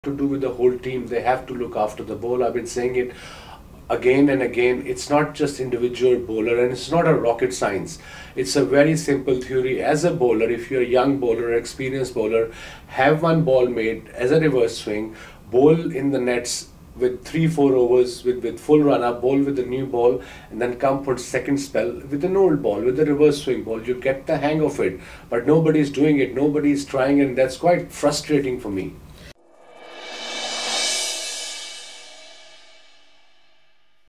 Wasim Akram - Absence of reverse swing
Wasim Akram laments the complete absence of reverse swing in the modern game.